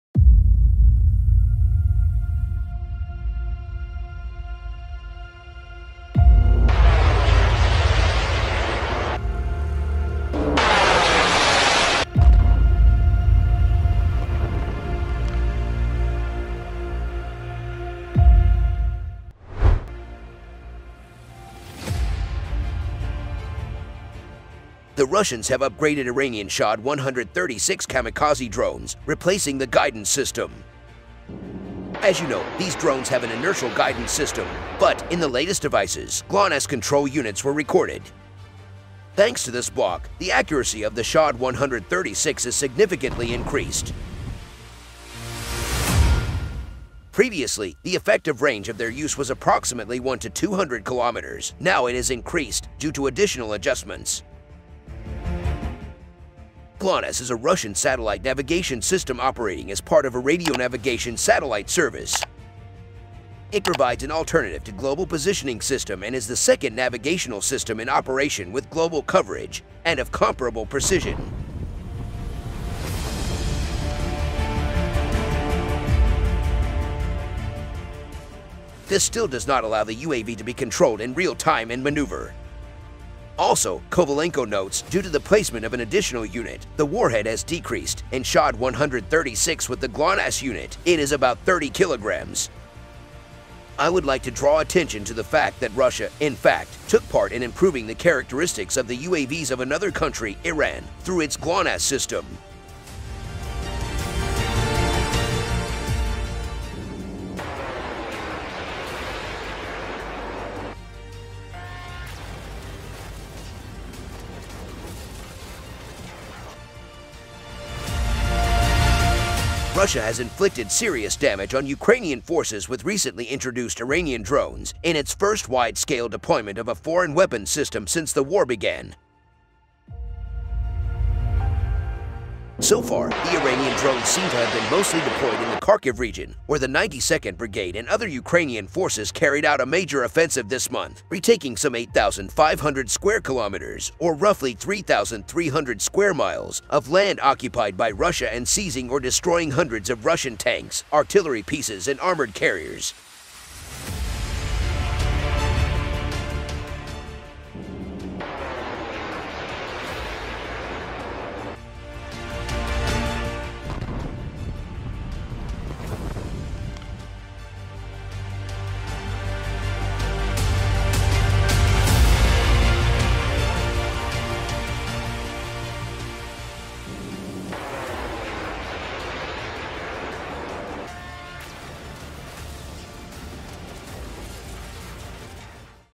Shahed-136 Kamikaze Drones Upgrade From Russia
Shahed-136 Mamikaze Drones have an inertial guidance system. But, in the latest devices, GLONASS control units were recorded. Thanks to this block, the accuracy of the” Shahed-136“is significantly increased. Previously, the effective range of their use was approximately 170-200 km, now it is increased, due to additional adjustments.

Russia has inflicted serious damage on Ukrainian forces with recently introduced Iranian Shahed-136 drones, in its first wide-scale deployment of a foreign weapons system since the war began.